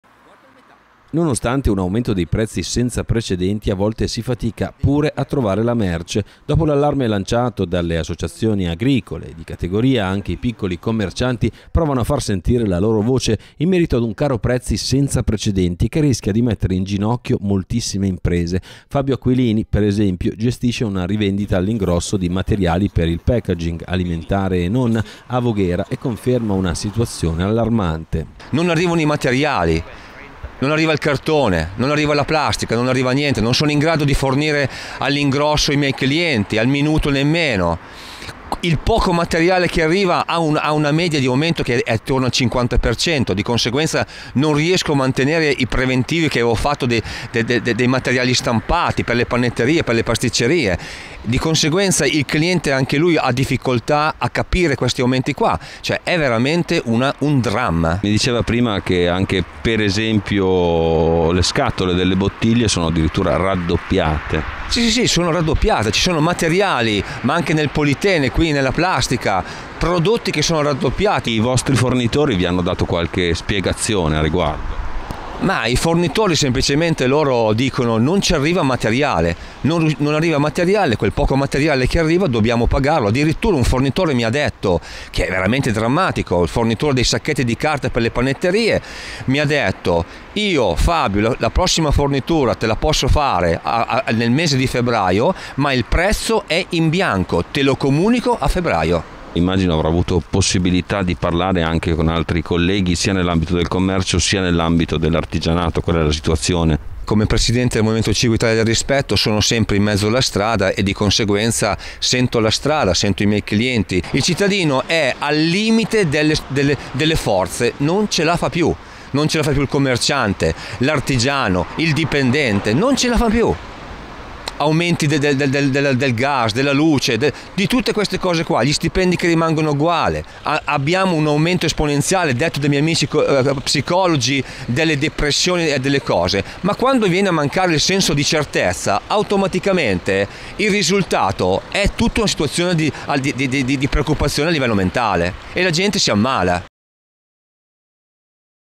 Un 'caro' Natale, commercianti senza merce o con prezzi raddoppiati: così saremo presto in ginocchio
Nonostante un aumento dei prezzi senza precedenti, a volte si fatica anche a trovare la merce. Dopo l’allarme lanciato dalle associazioni agricole, anche i piccoli commercianti provano a far sentire la loro voce in merito a un caro prezzi senza precedenti che rischio di mettere in ginocchio centinaia di imprese. Fabio Aquilini, per esempio, gestisce una rivendita all’ingrosso di materiali per il packaging, alimentare e non, a Voghera e conferma una situazione allarmante.